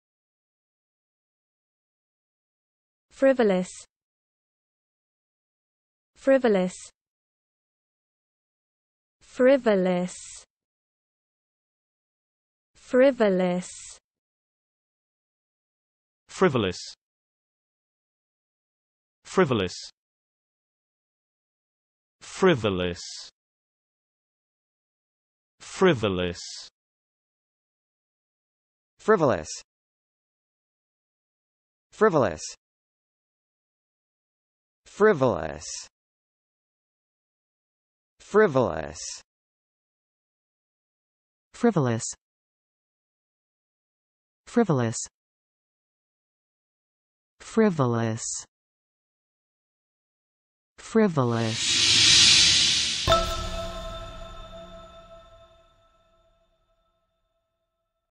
How to Pronounce "Frivolous"
Need help to learn English? Find the best deals on the best English courses

Thanks for viewing our video on how to say "Frivolous".

Phonetic spelling: ˈfrivələs.

1. Frivolous is used as an Adjective.
Definition: not having any serious purpose or value.
Example: rules to stop frivolous lawsuits
Words related to "Frivolous":

Do you have a better definition for "Frivolous"? Drop a note about Frivolous in the comments.

Fancy submitting your own word to pronounce.tv?